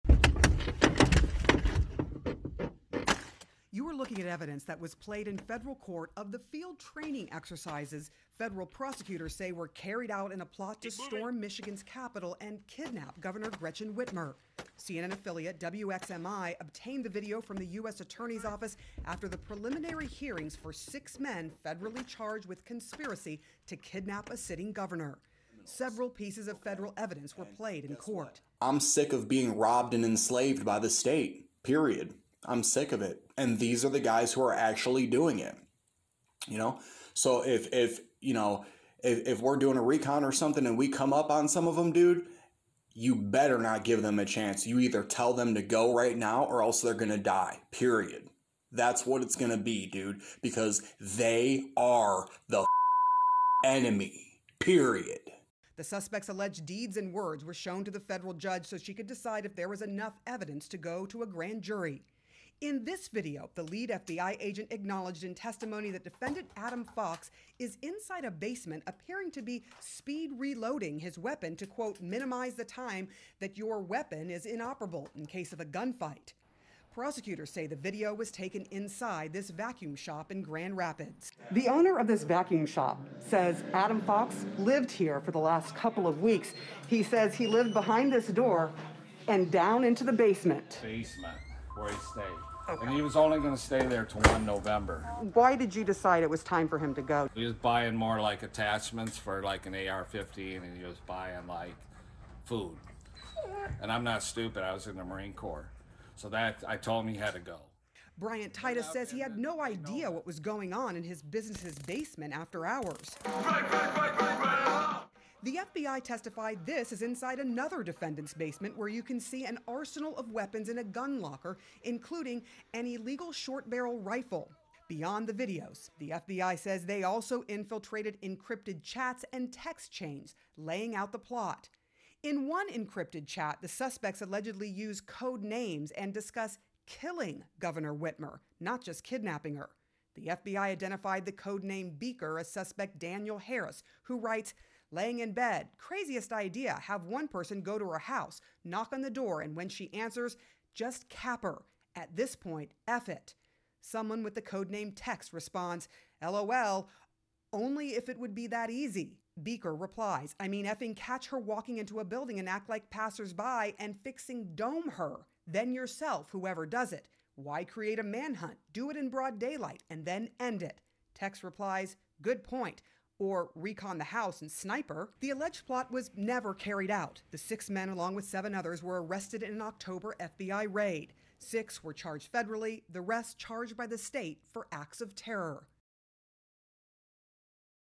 NEW VIDEO | Suspects charged in plotting kidnapping of Michigan governor shown training
New details are emerging about an alleged plot to kidnap Michigan’s governor.

The state attorney general's office says a man from Wisconsin is now the fourteenth suspect to face charges.

And, hearings are underway for six men with federal charges against them.

And new video and other evidence produced for those court proceedings appear to show the suspects plotting.

You are looking at evidence that was played in federal court of the field training exercises federal prosecutors say were carried out in a plot to storm Michigan’s capital and kidnap Governor Gretchen Whitmer.